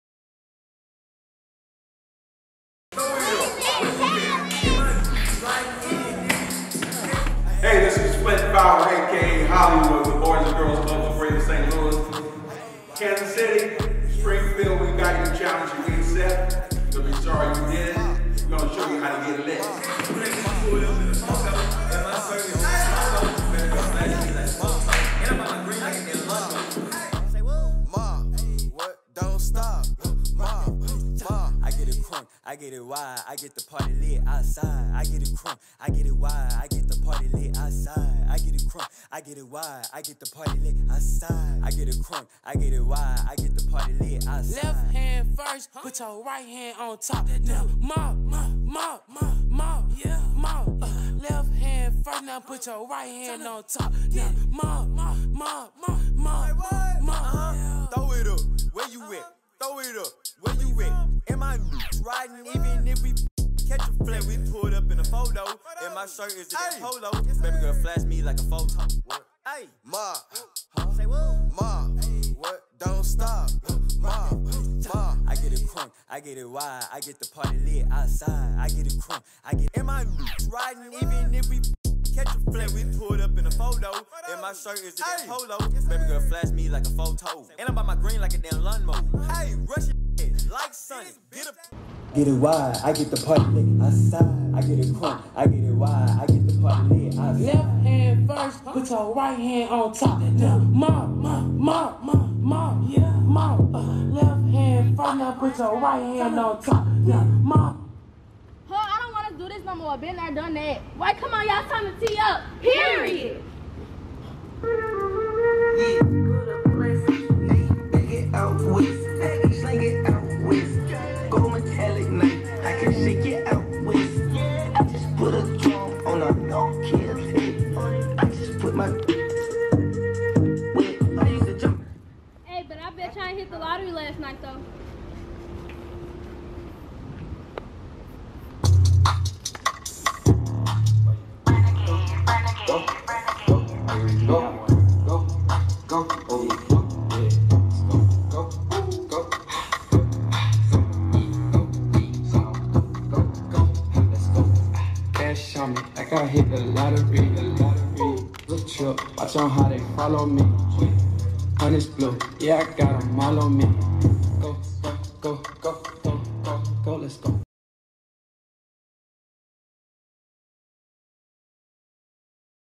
Dance Off Challenge
Challenge accepted! Our friends at the Boys & Girls Clubs of Greater Kansas City and Boys & Girls Clubs of Springfield challenged us to a danceoff! You're up next Boys & Girls Clubs of Greater Cincinnati & Boys & Girls Clubs of Columbia MO! It's your turn to get lit!!